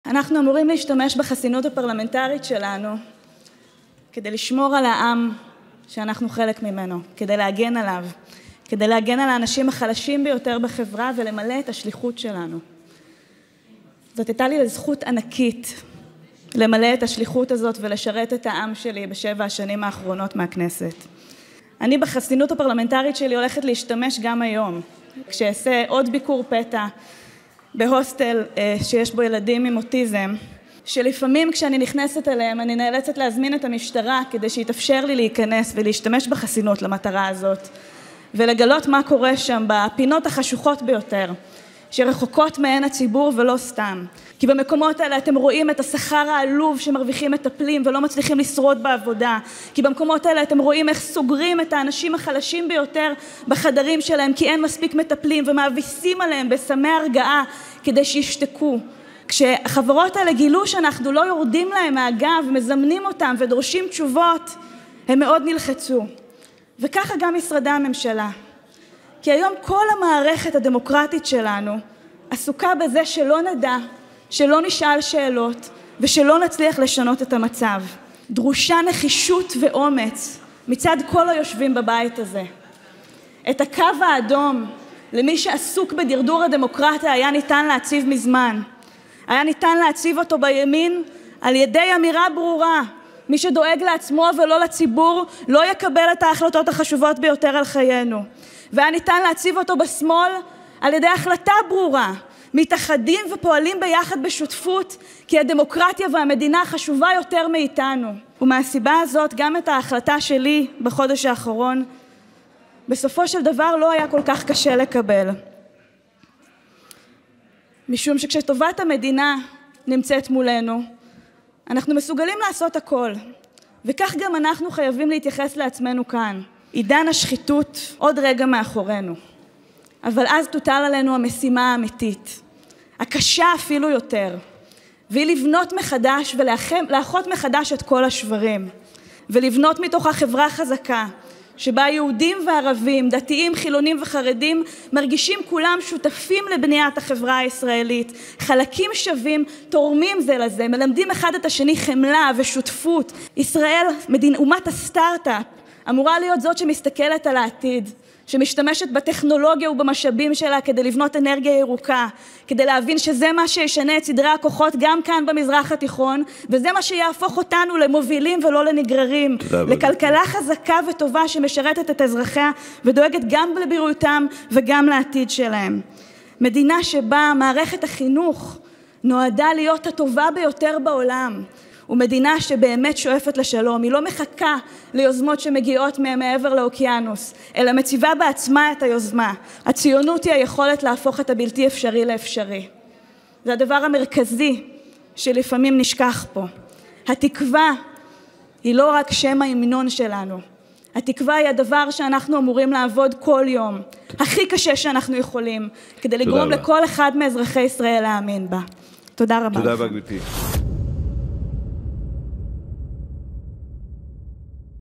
מאחורי ההצגות והמסכות של הפוליטיקה
מאחורי ההצגות והמסכות של הפוליטיקה - יש חיים אמיתיים. אנשים אמיתיים שצריכים להתמודד עם אתגרים אמיתיים ושזקוקים למדינה אמיתית שתסייע. כבר שנים שהאזרחים לא נמצאים בכלל בתוך הדיון הפוליטי. התקופה החשוכה הזו תחלוף תכף - ואחריה העבודה שלנו תהיה אפילו יותר קשה.

נפרדתי כך מהכנסת - לעת עתה. בלי לשכוח להשתמש בחסינות שלי (אבן יסוד בדמוקרטיה!) גם בחודש האחרון - אבל תכף תשמעו בשביל מה